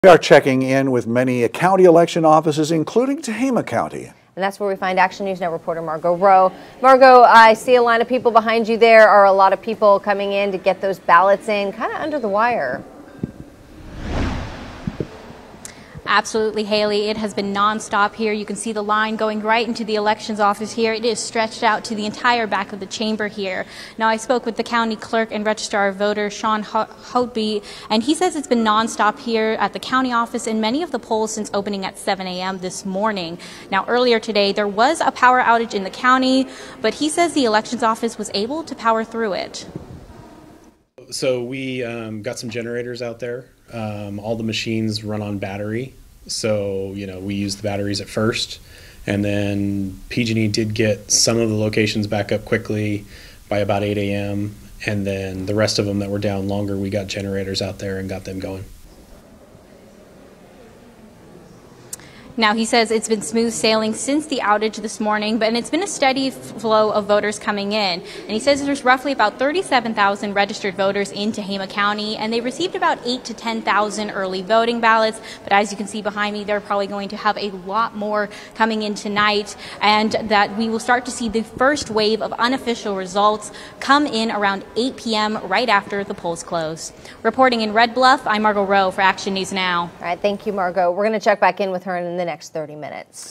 Voters casting their ballots in Tehama County
County Clerk and Registrar of Voters Sean Houghtby said it has been non-stop since opening at 7 a.m.